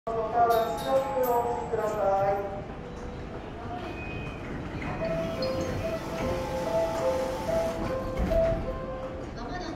新小岩駅ホームドア閉扉×E235系1000番台グリーン車
一番列車を誰もいないであろう新小岩駅で補足しました（同じこと考えてる人が動画上げてたけど）

いろいろ撮ろうとしてたから動画がうろうろしてますがゆるして（こんど撮り直す）